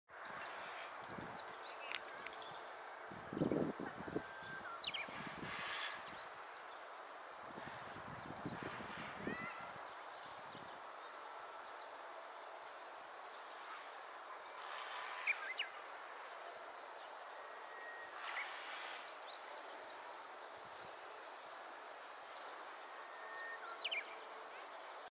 ウェザーリポート動画0406@大阪枚方 14時30分頃（キジ・雌）
キジの女子はあまり姿を見せてくれませんが、今日はじっくり見られました。